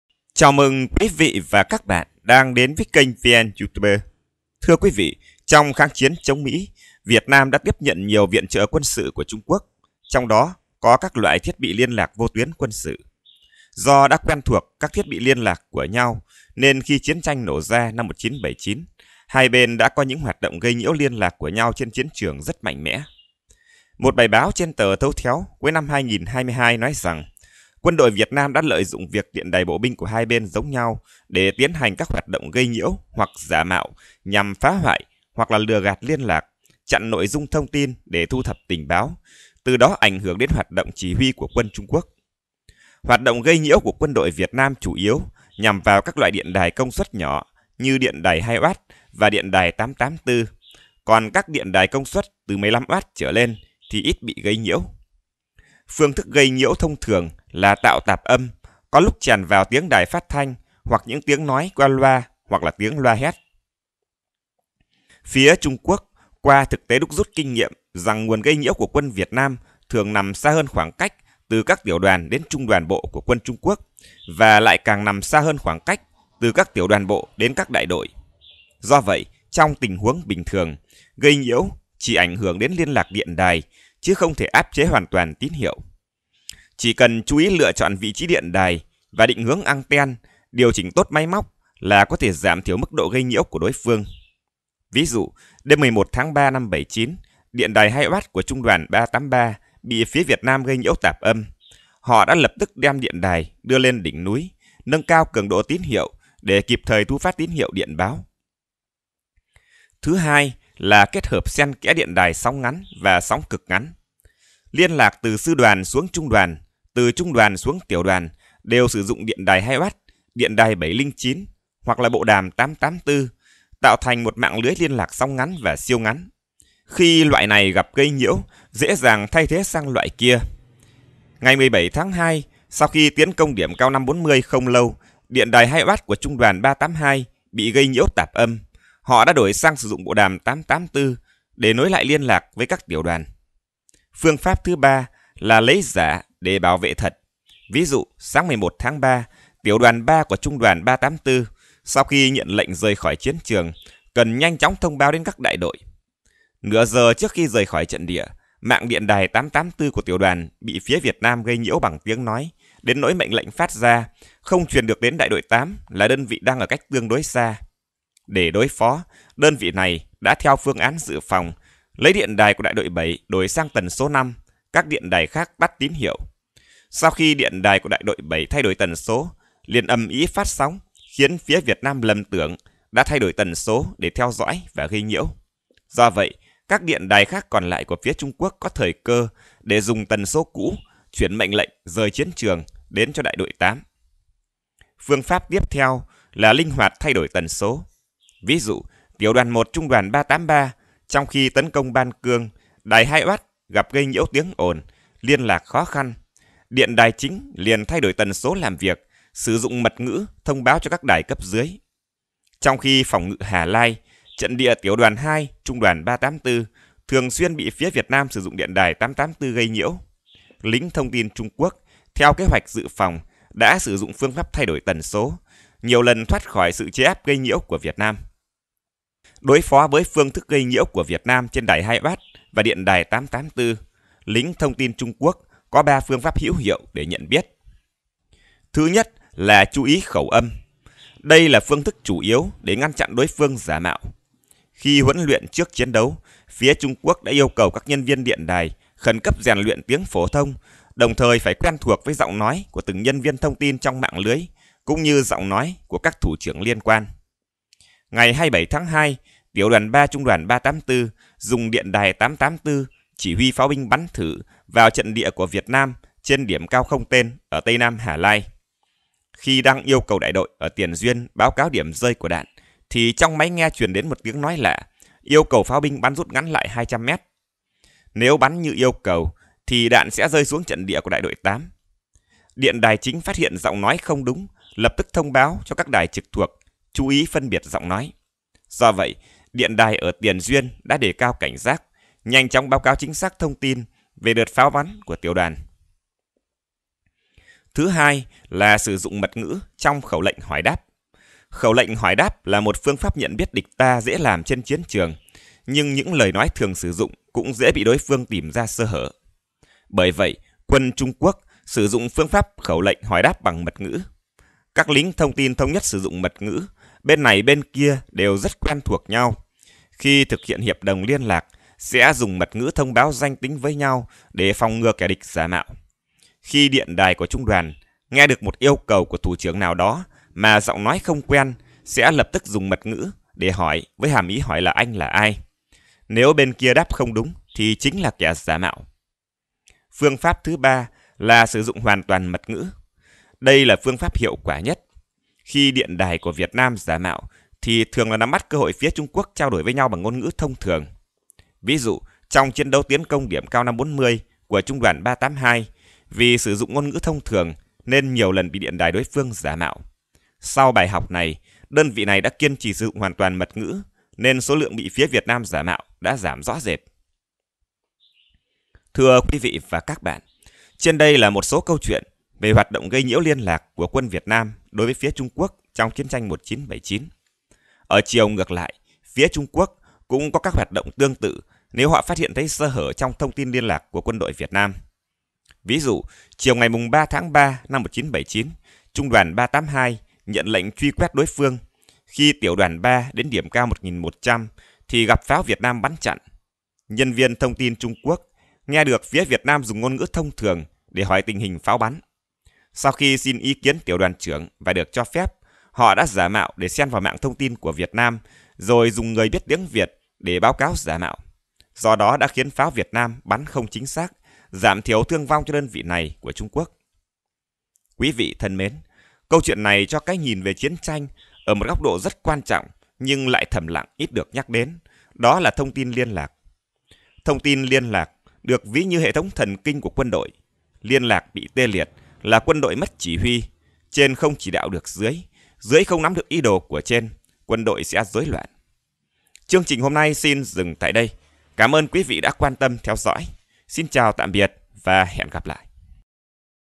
Kể chuyện quân đội VN dùng điện đài TQ để lừa TQ
Do sự tương đồng về trang bị thông tin liên lạc nên trong chiến tranh Việt - Trung 1979, hai bên đã có những hoạt động gây nhiễu vào hệ thống chỉ huy liên lạc vô tuyến của nhau rất mạnh mẽ.